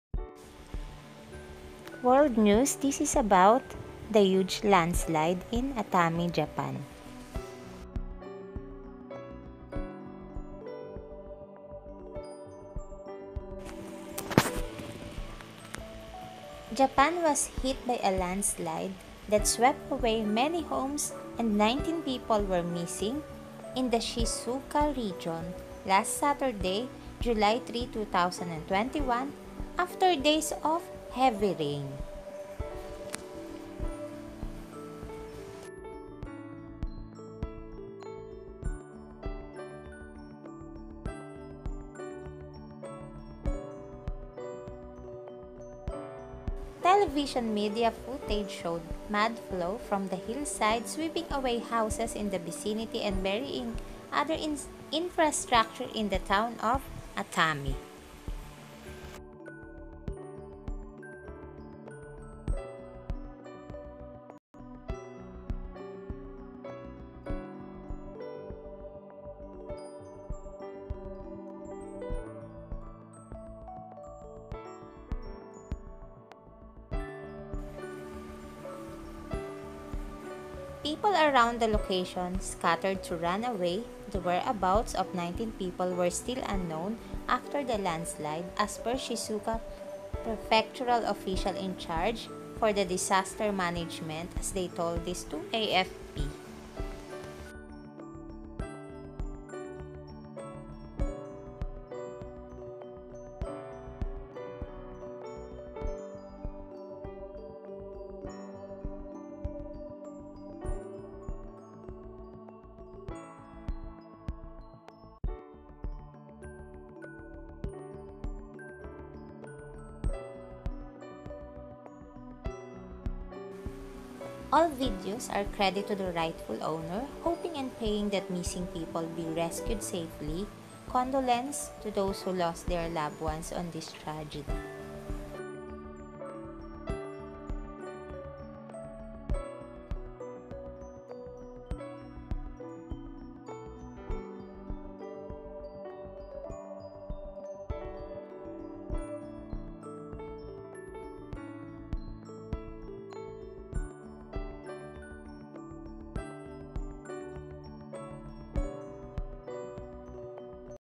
July 3, 2021 |Residents Missing After Landslide in Japan
Atami, Japan Landslide | A Very Destructive Landslide hit Atami, Shizuoka, Japan Last July 3, 2021
Japan was hit by a landslide that swept away many homes and 19 people were missing in the Shizuoka region last Saturday, July 3, 2021 after days of heavy rain.
Television media footage showed mudflow from the hillside sweeping away houses in the vicinity and burying other infrastructure in the town of Atami.  Then, the people around the location scattered to run away.  "The whereabouts of 19 people are unknown", after the landslide, a Shizuoka prefectural official in charge of disaster management told AFP.